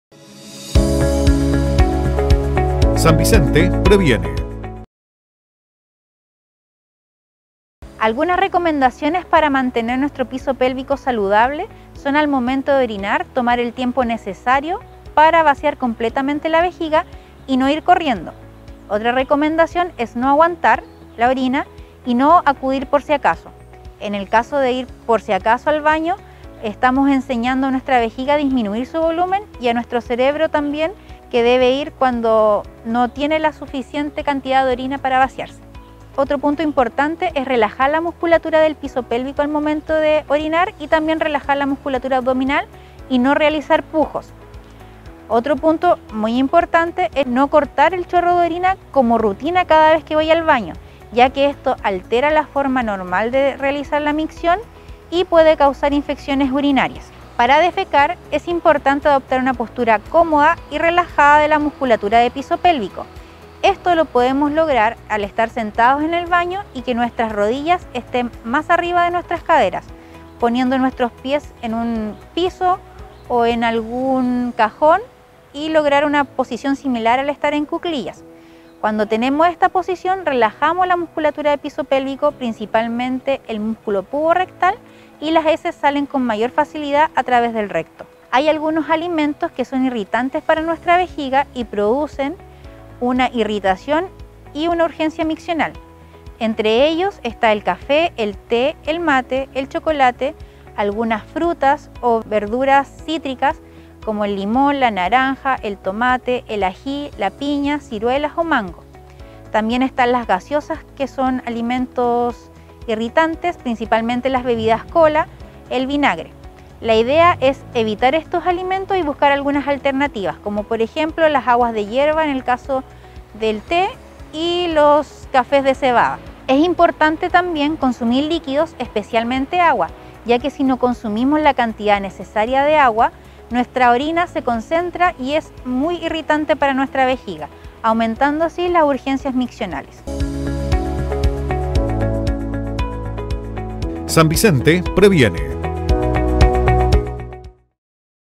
RECOMENDACIONES PARA MANTENER UN PISO PÉLVICO SALUDABLE
Continuando con la campaña educativa del Programa MAS Adultos Mayores Autovalentes, en el contexto del mes de las personas mayores, la kinesióloga especialista en piso pélvico, Rosemery Jorquera, entregó algunas recomendaciones para mantener un piso pélvico saludable.

Entre ellas, mencionó tomar el tiempo necesario para orinar, no aguatar ni ir al baño “por si acaso”, relajar la musculatura de piso pélvico y abdominal, no pujar y no cortar el chorro de orina como una rutina.

También aconsejó evitar alimentos que son irritantes para la vejiga y que producen urgencia miccional, como café, té , mate, chocolate, alimentos cítricos, gaseosas, vinagre; así como consumir abundantes líquidos, especialmente agua.